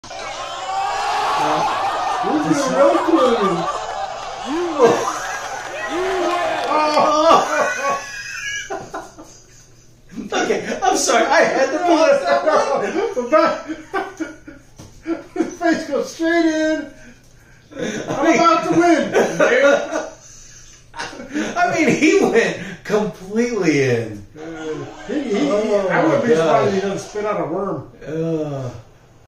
Bicycle wreck short
Mike & Steve react to a video of a bicyclist that goes face first into the mud